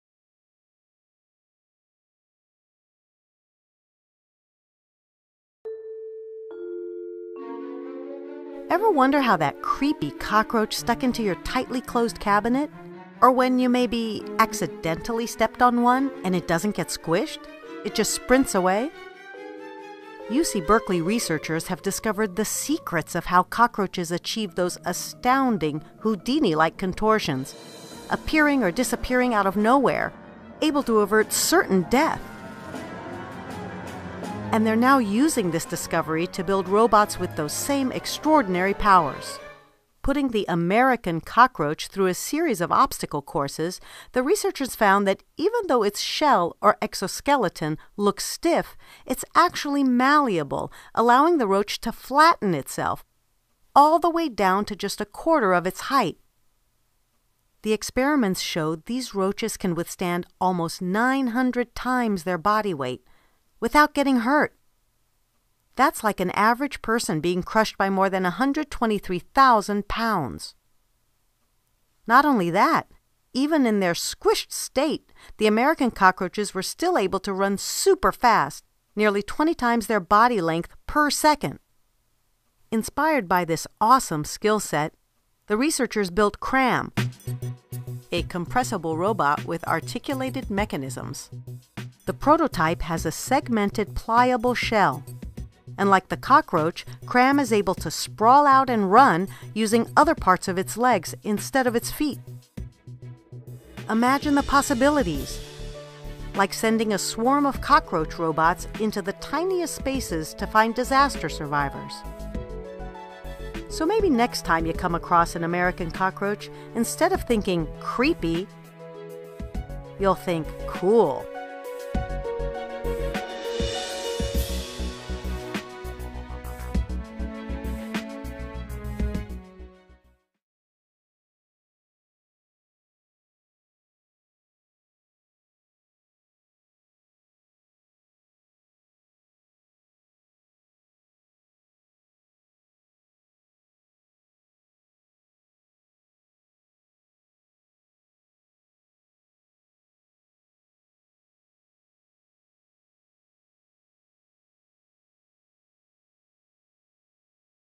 Meet CRAM: The Advanced Robotic Cockroach
Periplaneta americana, commonly known as the American cockroach, one of the most dreaded pest of them all, has now crept its way into robotic studies, thankfully far from the reaches of our homes! Researchers at UC Berkeley noted the ability of cockroaches to squeeze and maneuver them-selves into the tiniest of cracks and crevices: under doors, into cabinets and between walls, all in a matter of seconds.

The CRAM robot, inspired by cockroaches, can crawl even when squashed to half its size.